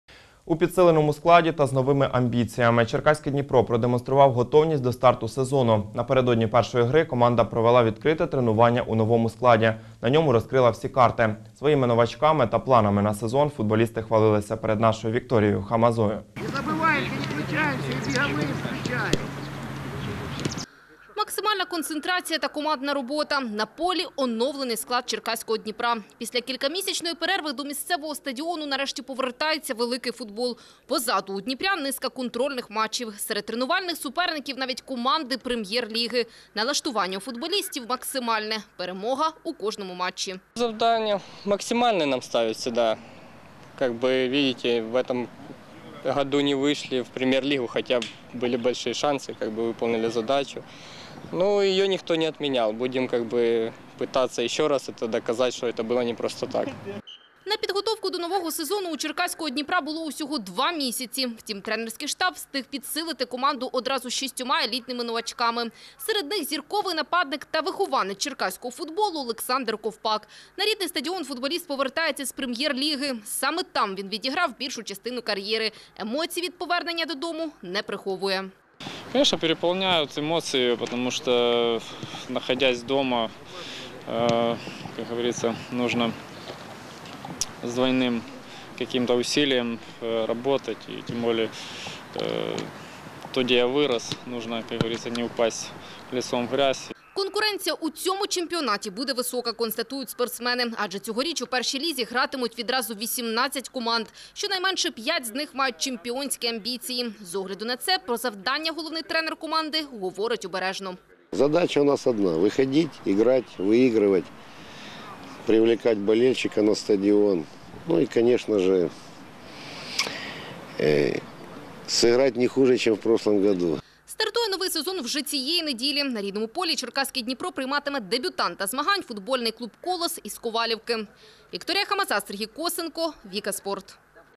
«Черкаський Дніпро» демонструє готовність до старту сезону
Напередодні першої гри команда провела відкрите тренування у новому складі